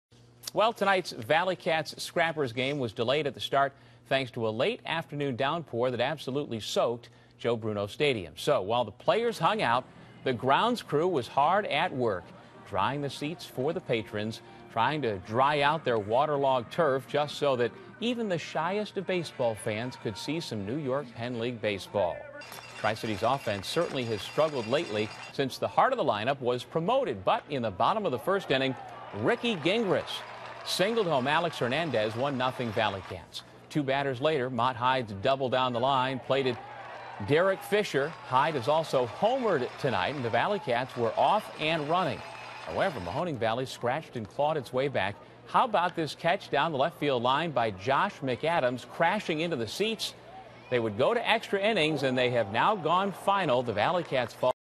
Scrappers 7, ValleyCats 6 in 11
Troy, NY - In a game that was delayed at the start thanks to rain, the Tri-City ValleyCats and Mahoning Valley Scrappers wound up playing extra innings.  In the end of a long Thursday night at Joe Bruno Stadium, the visitors from Ohio prevailed, 7-6 in 11 innings.
The ValleyCats will look to avoid a series sweep Friday night.